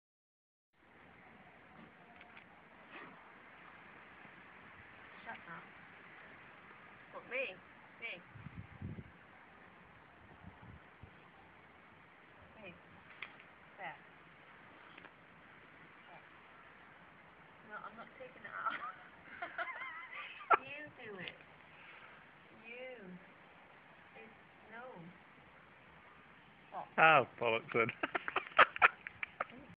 Open the bag lady !!
This chimp wants to see inside the handbag and gives clear instructions before giving up in disgust